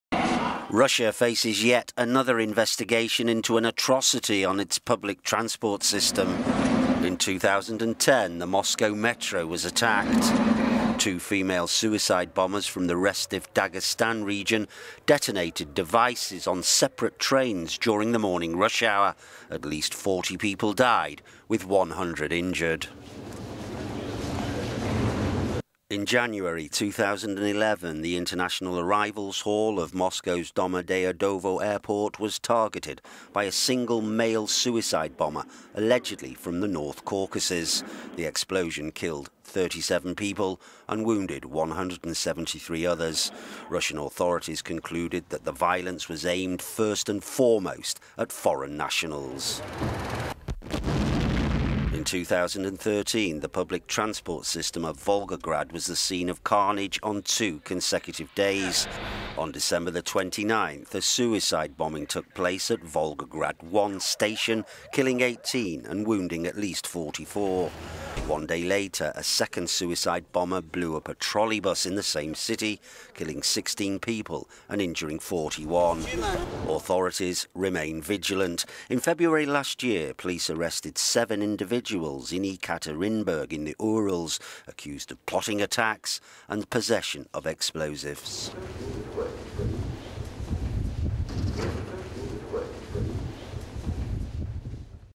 Russia's transport system a target for terrorism
Russia faces yet another investigation into an atrocity on its public transport  system.

In 2010 the Moscow metro was attacked

Two female suicide bombers from the restive Dagestan region detonated devices on separate trains during morning rush hour at least 40 people died with 100 injured.

5 years ago... Terrorist attack in the Moscow metro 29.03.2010 Nikita Parshukov (@SNParshukov) March 29, 2015

In January 2011 the international arrival hall of Moscow's…